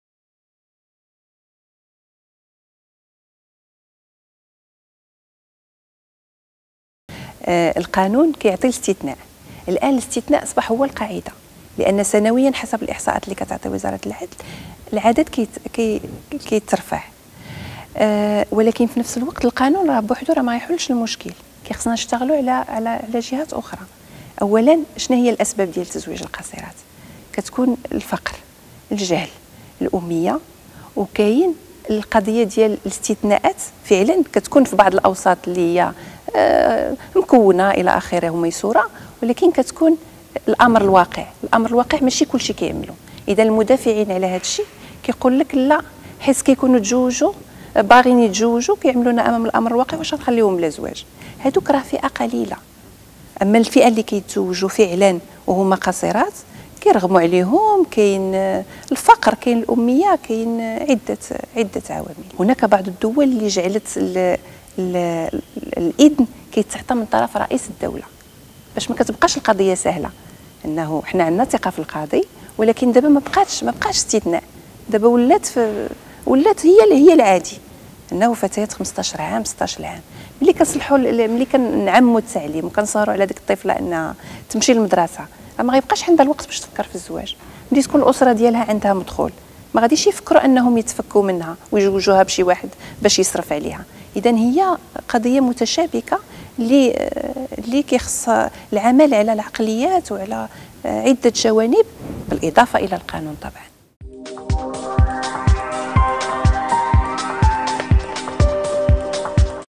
المساواة بين الجنسين وتزويج القاصرات - نعيمة بنيحيى
" بعض الدول تزوج القاصرات بإذن من رئيس الدولة" نعيمة بنيحي برلمانية عن حزب الاستقلال